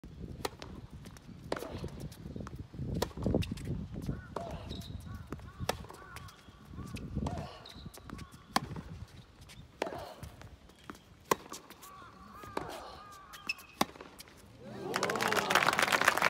広角に攻め込む パワーストローク【ITF横浜慶應2023/F】清水綾乃(JPN)
横浜慶應チャレンジャー国際テニストーナメント2023 supported by 三田興産
ITF女子4万ドル
シングルス決勝戦
Aliona FALEI 63 75 清水綾乃(JPN)